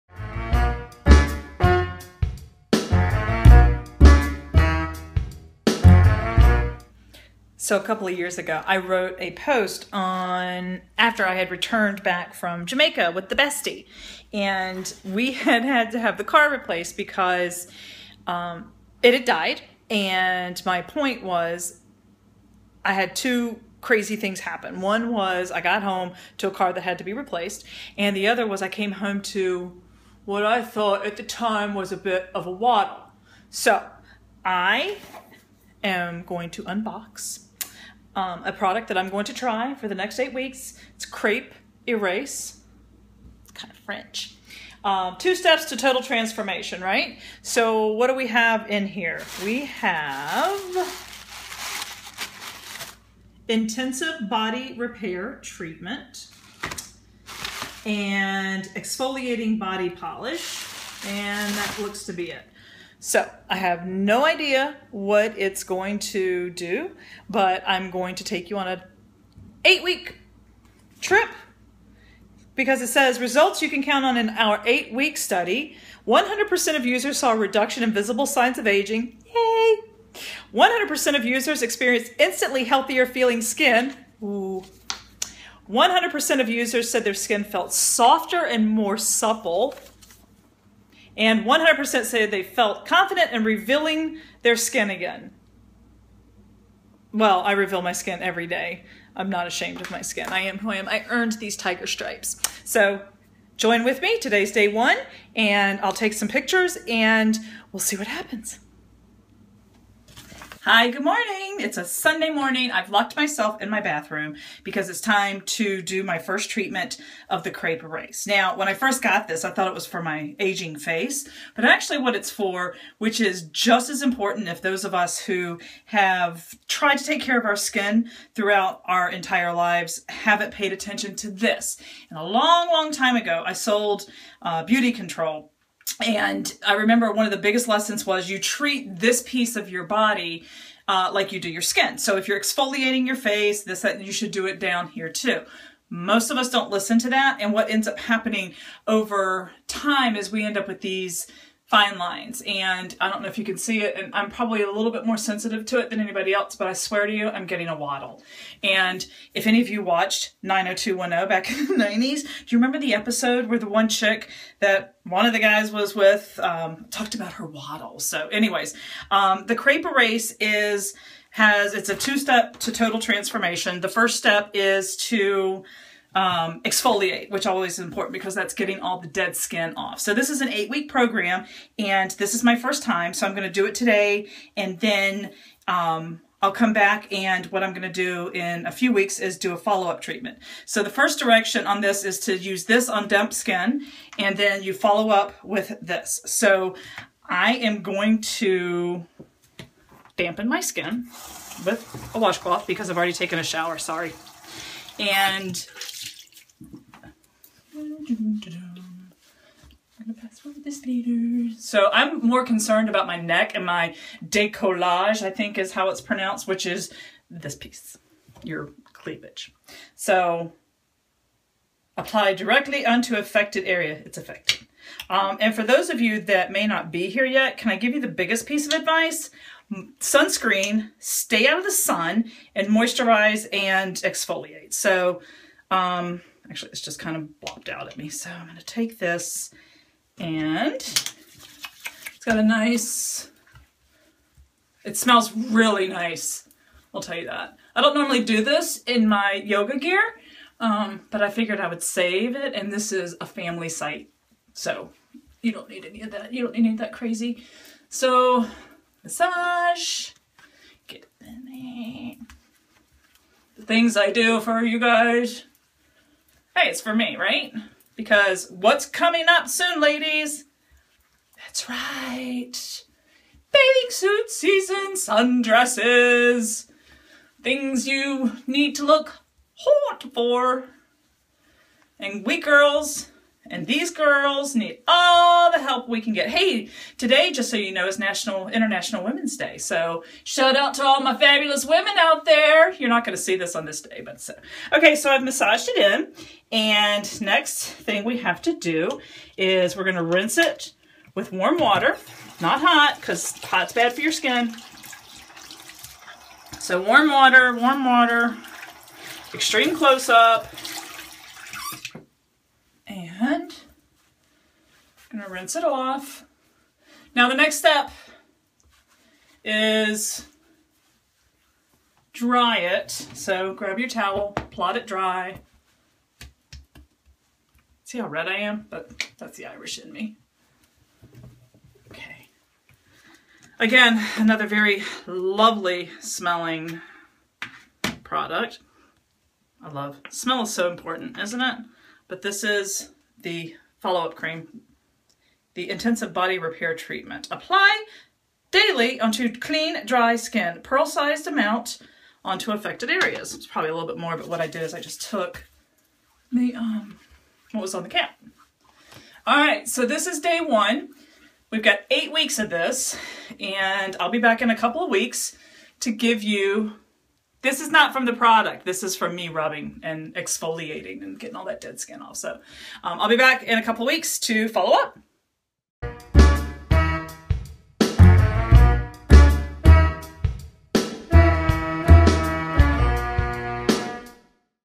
Crepe Erase First Impressions
Oh the skin once it needs help... We're testing out Crepe Erase to see if it helps reverse the damage of sun and life.